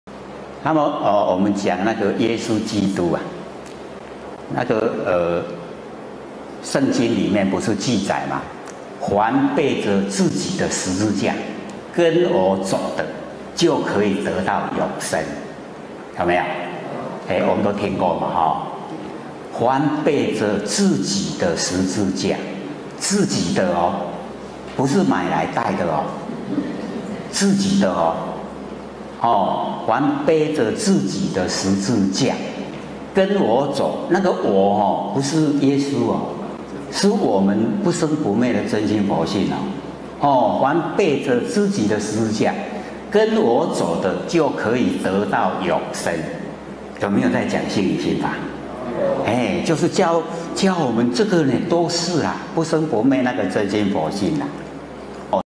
聖經 彌勒佛 濟公活佛 性理心法 明心見性 超越萬八 性理心法班 成佛班 劉芳村 講經說法 李氏壇 明德
耶穌基督---聖經
   我們講耶穌基督，聖經裡面不是記載嗎？「凡背著自己的十字架，跟我走的，就可以得到永生」，有没有？我們都聽過嘛！凡背著自己的十字架，自己的喔！不是買來戴的，跟我走的，那個我不是耶穌喔，是我們不生不滅的真心佛性，有没有在講性理心法。